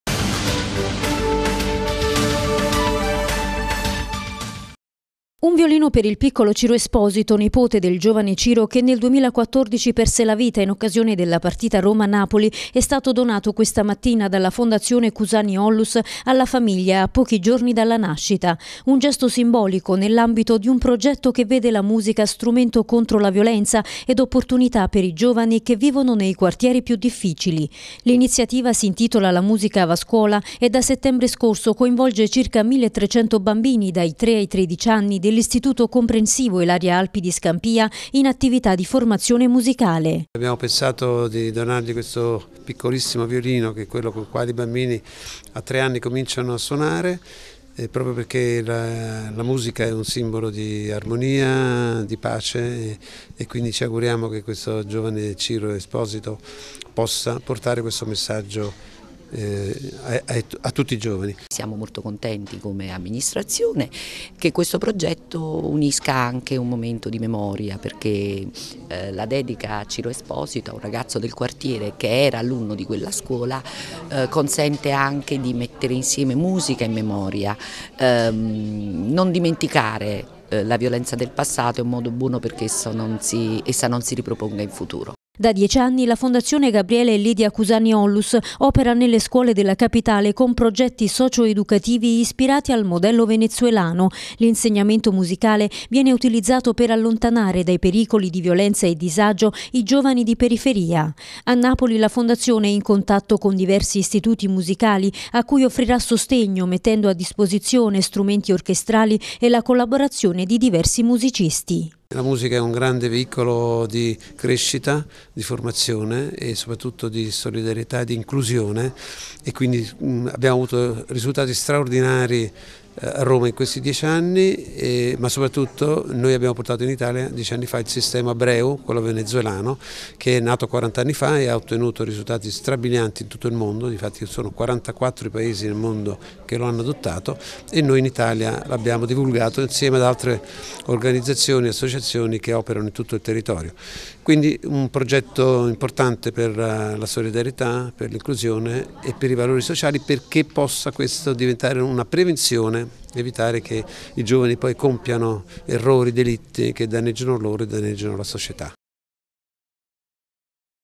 Musica contro la violenza, un violino per il nipote di Ciro Esposito
Il nipote di Ciro Esposito, a pochi giorni dalla nascita, riceve in dono un violino dalla Fondazione Cusani Onlus. E' il progetto 'La musica va a scuola' promosso nell'I.C. Ilaria Alpi di Scampia per allontanare i giovani di periferia da situazioni di violenza e disagio.